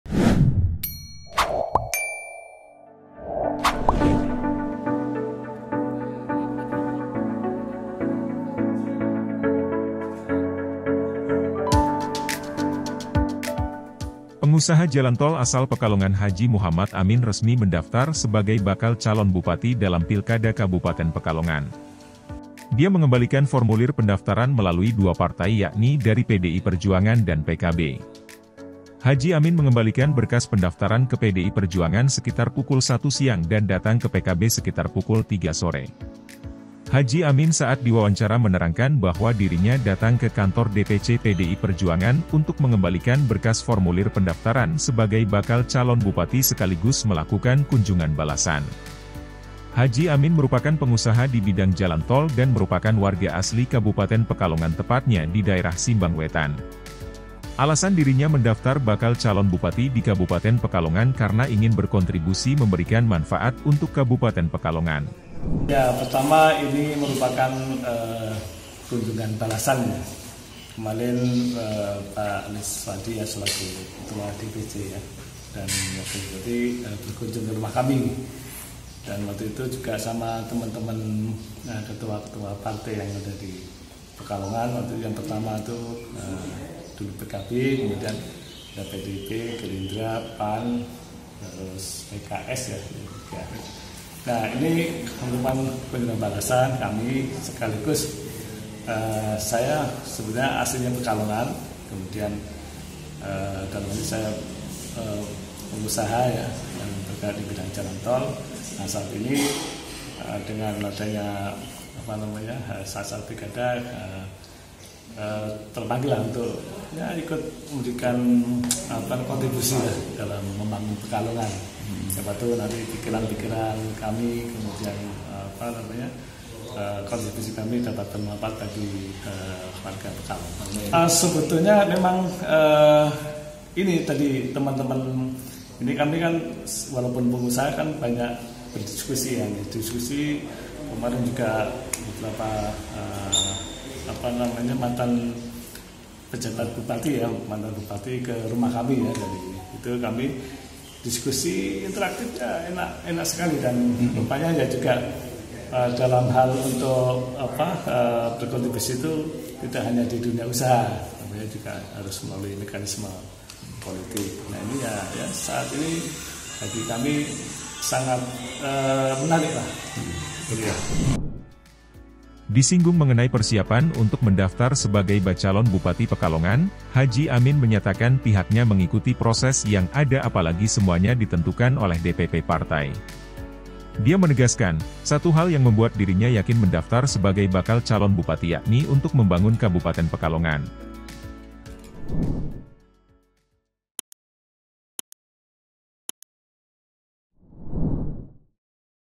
Pengusaha Jalan Tol H. M. Amin Daftar Bacalon Bupati Pekalongan Lewat PDI-P dan PKB
Pengusaha Jalan Tol asal Pekalongan Haji Muhammad Amin resmi mendaftar sebagai bakal calon bupati dalam Pilkada Kabupaten Pekalongan. Dia mengembalikan formulir pendaftaran melalui 2 partai yakni dari PDI Perjuangan dan PKB.

Haji Amin mengembalikan berkas pendaftaran ke PDI Perjuangan sekitar pukul 1 siang dan datang ke PKB sekitar pukul 3 sore. Haji Amin saat diwawancara menerangkan bahwa dirinya datang ke Kantor DPC PDI Perjuangan untuk mengembalikan berkas formulir pendaftaran sebagai bakal calon bupati sekaligus melakukan kunjungan balasan.

Haji Amin merupakan pengusaha di bidang jalan tol dan merupakan warga asli Kabupaten Pekalongan tepatnya di daerah Simbang Wetan. Alasan dirinya mendaftar bakal calon bupati di kabupaten Pekalongan karena ingin berkontribusi memberikan manfaat untuk Kabupaten Pekalongan.